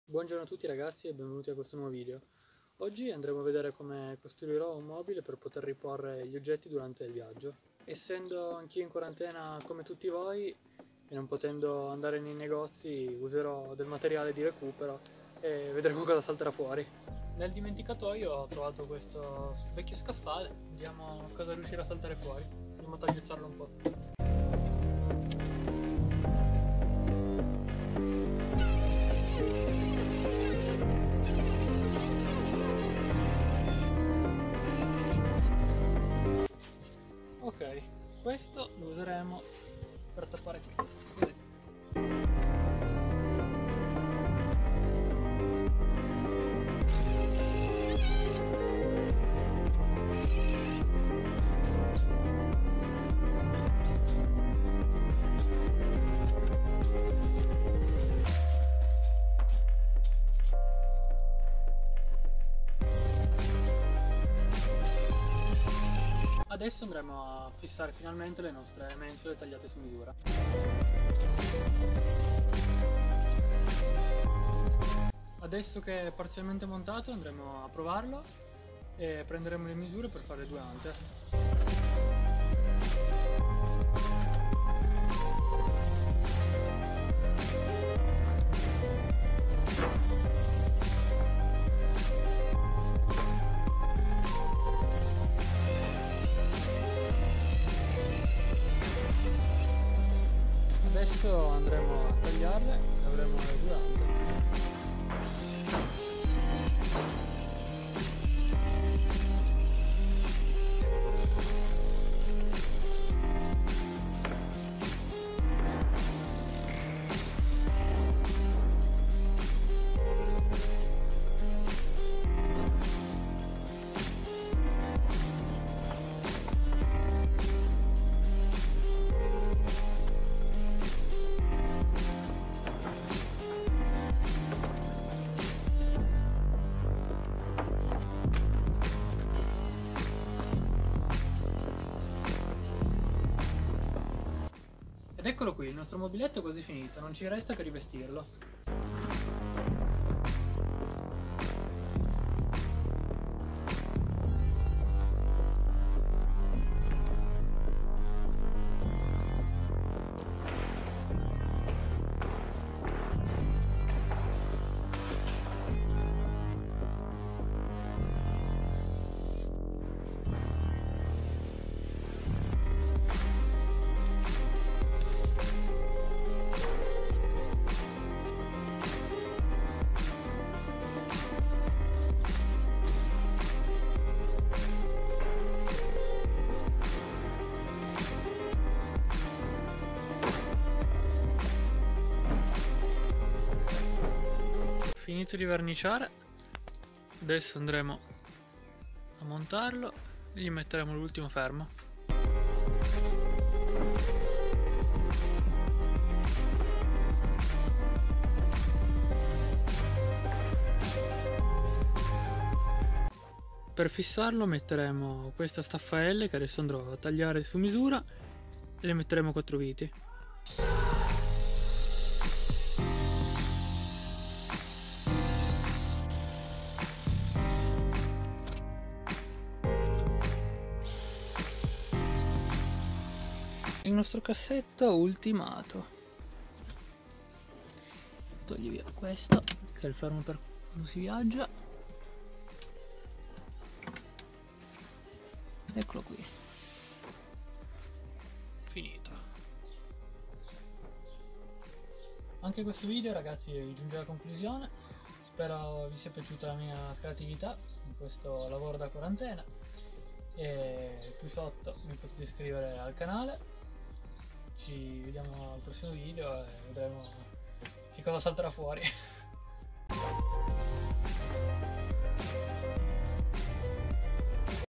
DA UN MOBILE DIMENTICATO A UN MOBILE DA VIAGGIO PER SKYTROLL-LAVORI DA QUARANTENA -HOME MADE!AWESOME
Buongiorno a tutti ragazzi,un benvenuto ai nuovi iscritti e un saluto a chi mi segue già da un po', oggi andremo a vedere come ho costruito in questa quarantena un mobile per potere mettere materiale aggiuntivo durante i prossimi viaggi, siamo passati da un vecchio mobile in disuso a un bellissimo mobile autocostruito .
Spero di essere di aiuto e dello spunto a chi non sa' cosa fare, incentivando ancora di più l'home made!!!
Per seguire le mie avventure cercami anche sui vari social:
Buona visione ragazzi e al prossimo video!!!